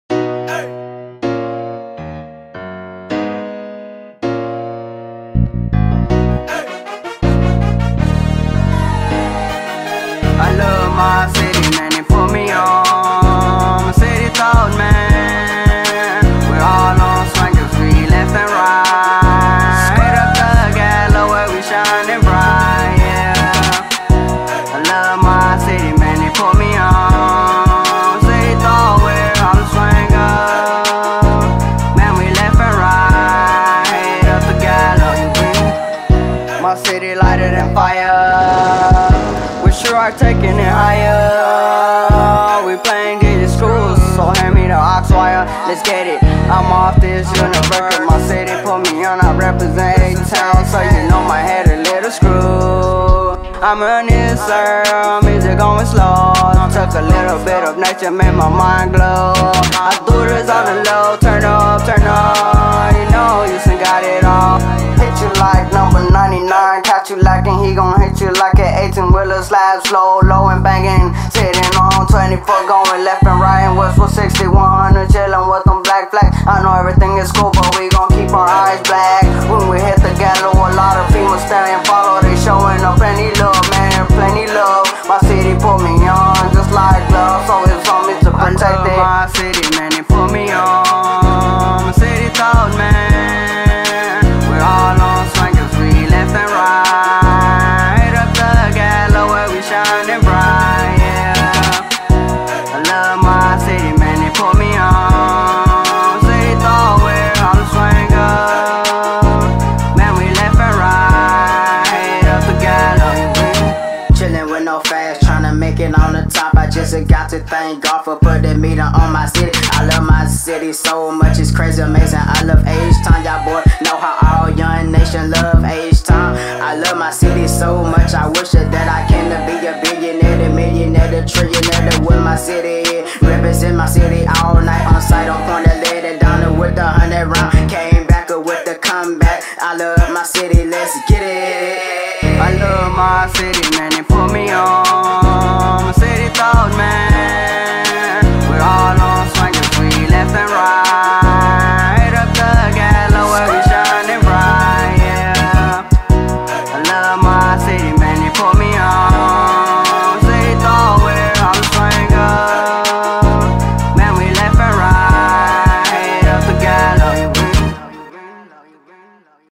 Love My City
Workshop Houston has four shops that provide resources and support for young people: the Chopper Shop (welding and metal fabrication), the Beat Shop (hip-hop music production), the Style Shop (fashion design) and the Scholar Shop (tutoring and academic enrichment).
Workshop Houston's mission is to provide youth with creative, technical and educational resources. Our vision is to lay the groundwork for a just society by creating a community that provides youth with support, expanded opportunities and alternative definitions of success.